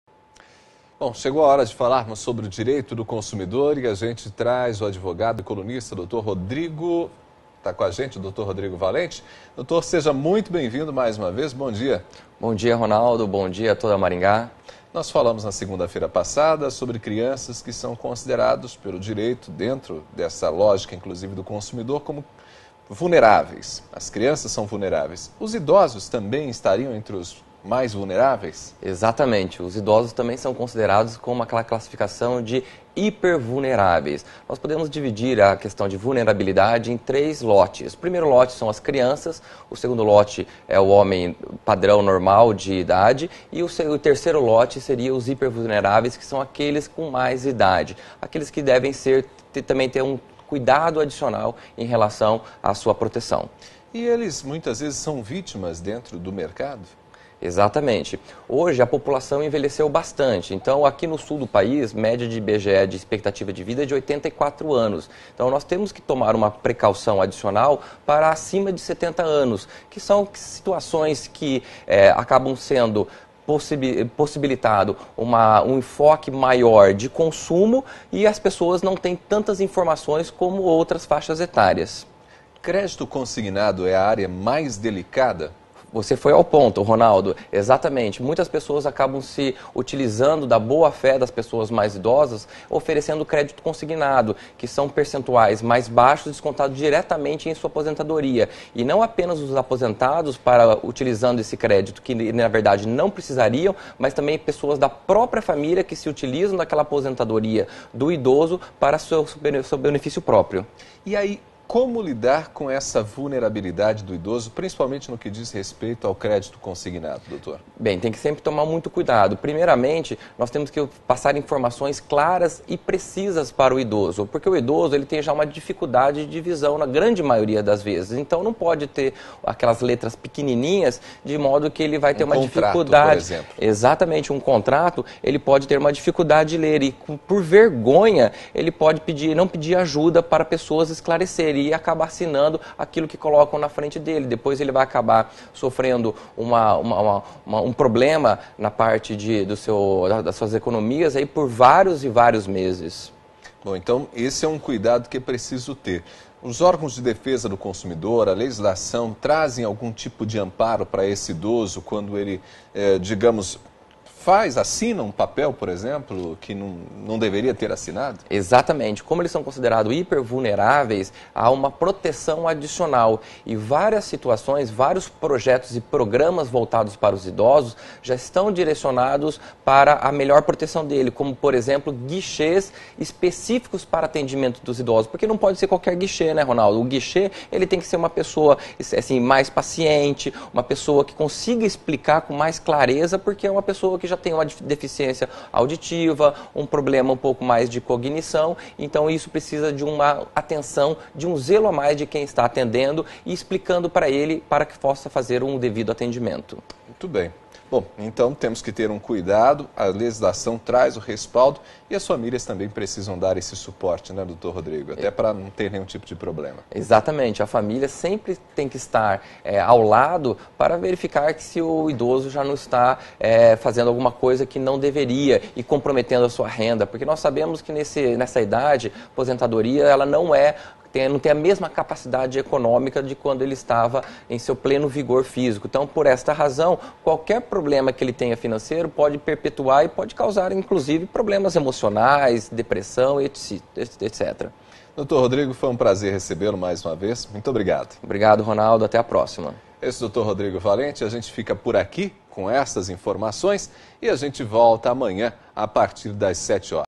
Entrevista com o advogado Rodrigo Valente
Tem idosos na família?
O advogado e nosso colunista Rodrigo Valente fala sobre o direito do idoso como consumidor.